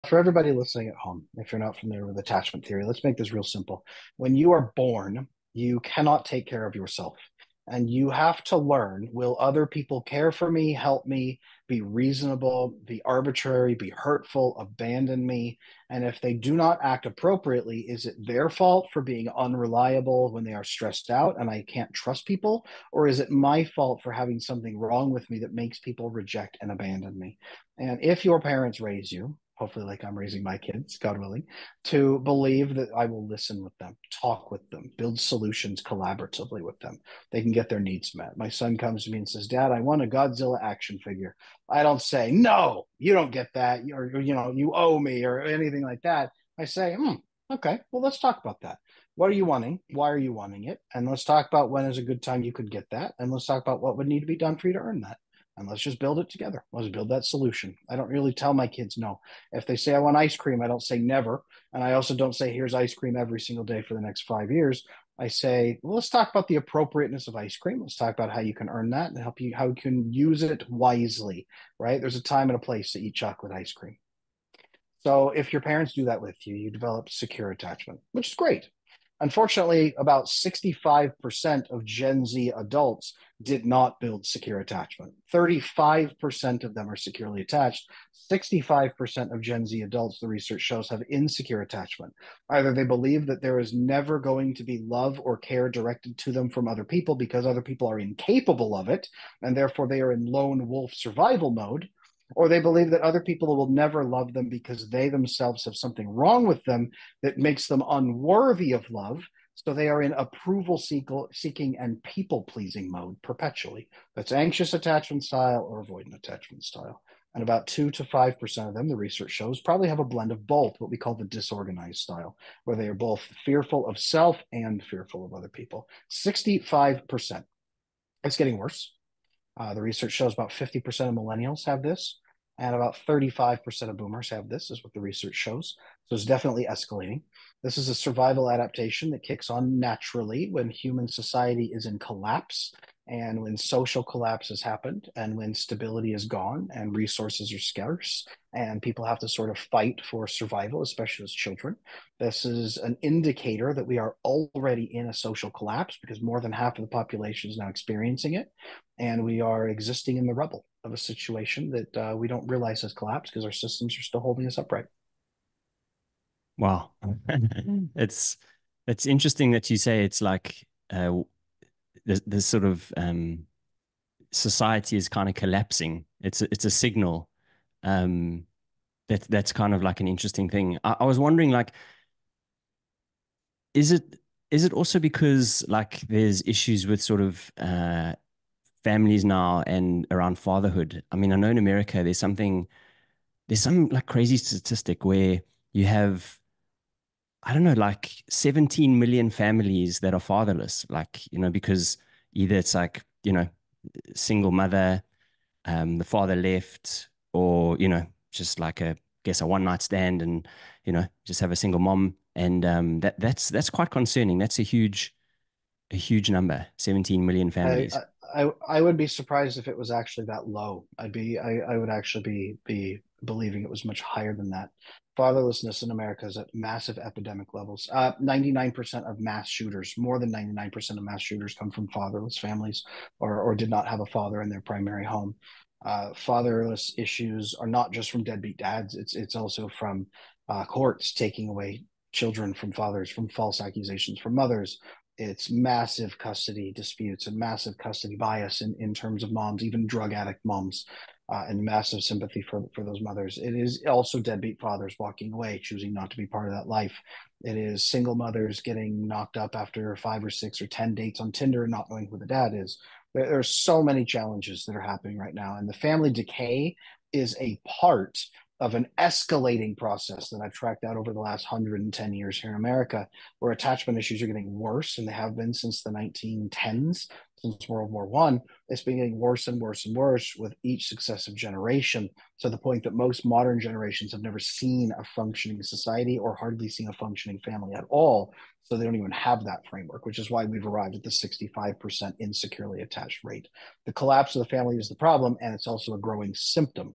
Understanding Your Crippling Attachment Issues!
Adam Lane Smith, described what Attachment Theory is and how it is holding your relationships back from thriving.

Research shows about 50% of millennials have Attachment Issues, and about 35% of boomers have it too!

Fatherlessness in America is also at epidemic levels and ruining the fabric of society

ADAM LANE SMITH

RIDICULOUSLY HUMAN SOCIAL MEDIA ACCOUNTS